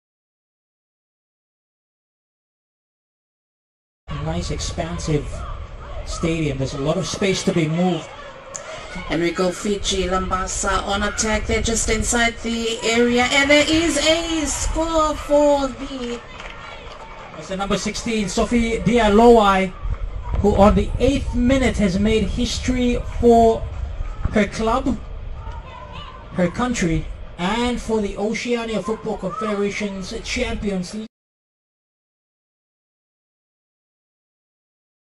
Sofi Diyalowai become first ever goalscorer at 2023 OFC Women's Champions League (Women’s Football)
Labasa Women FC and Fiji International Sofi Diyalowai becoming the first ever goalscorer at an 2023 OFC Women's Champions League inaugural. Fiji club Labasa Women FC registered an opening day victory over Samoan side Kiwi FC, 6-0.

Facebook Page: The News Of Oceania Football
Instagram: The News Of Oceania Football